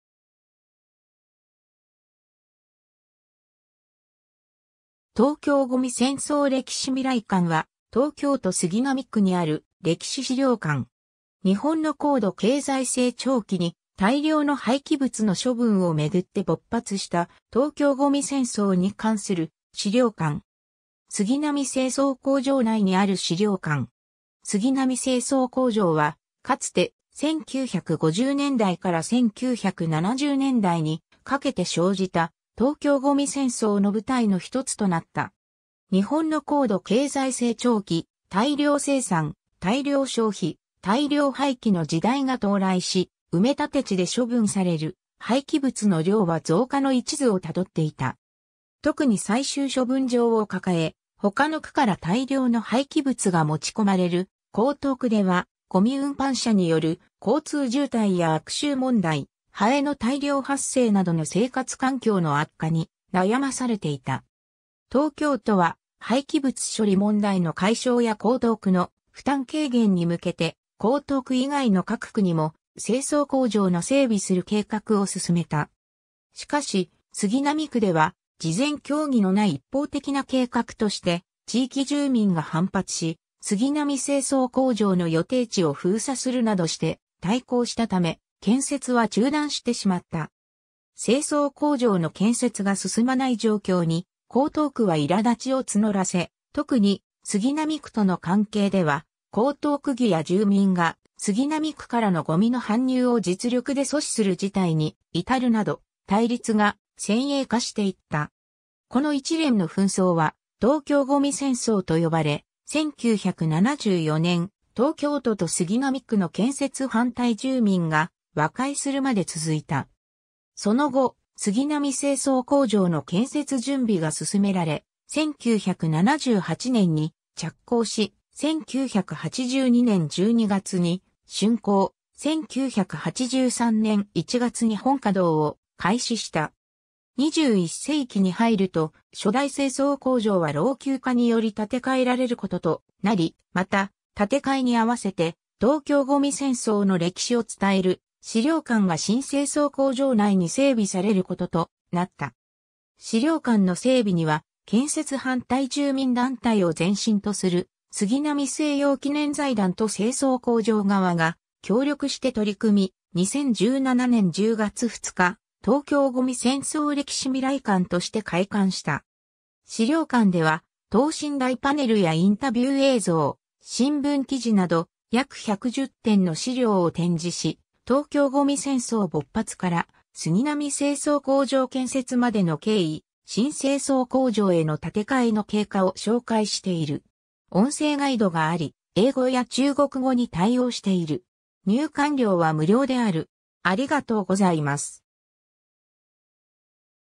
東京ごみ戦争歴史みらい館
東京ごみ戦争歴史みらい館 （とうきょう ごみせんそう れきしみらいかん）は、東京都杉並区にある歴史資料館。
日本の高度経済成長期に大量の廃棄物の処分をめぐって勃発した「東京ゴミ戦争」に関する資料館。
 杉並清掃工場内にある資料館。
杉並清掃工場は、かつて、1950年代から1970年代にかけて生じた「東京ゴミ戦争」の舞台のひとつとなった。
日本の高度経済成長期、大量生産・大量消費・大量廃棄の時代が到来し、埋立地で処分される廃棄物の量は増加の一途をたどっていた。
特に最終処分場を抱え、他の区から大量の廃棄物が持ち込まれる江東区では、ごみ運搬車による交通渋滞や悪臭問題、ハエの大量発生などの生活環境の悪化に悩まされていた。
 東京都は、廃棄物処理問題の解消や江東区の負担軽減に向けて、江東区以外の各区にも清掃工場の整備する計画を進めた。
しかし、杉並区では「事前協議のない一方的な計画」として地域住民が反発し、杉並清掃工場の予定地を封鎖するなどして対抗したため、建設は中断してしまった。
清掃工場の建設が進まない状況に江東区は苛立ちを募らせ、特に杉並区との関係では、江東区議や住民が杉並区からのゴミの搬入を実力で阻止する事態に至るなど、対立が先鋭化していった。
 この一連の紛争は「東京ゴミ戦争」と呼ばれ、1974年（昭和49年）、東京都と杉並区の建設反対住民が和解するまで続いた。
その後、杉並清掃工場の建設準備が進められ、1978年（昭和53年）に着工し、1982年（昭和57年）12月に竣工、1983年（昭和58年）1月に本稼働を開始した。
 21世紀に入ると、初代清掃工場は老朽化により建替えられることとなり、また、建替えに合わせて「東京ゴミ戦争」の歴史を伝える資料館が新清掃工場内に整備されることとなった。
 資料館の整備には、建設反対住民団体を前身とする「杉並正用記念財団」と清掃工場側が協力して取り組み、2017年（平成29年）10月2日、「東京ごみ戦争歴史みらい館」として開館した。
 資料館では、等身大パネルやインタビュー映像、新聞記事など約110点の資料を展示し、「東京ゴミ戦争」勃発から杉並清掃工場建設までの経緯、新清掃工場への建替えの経過を紹介している。
音声ガイドがあり、英語や中国語に対応している。
入館料は無料である。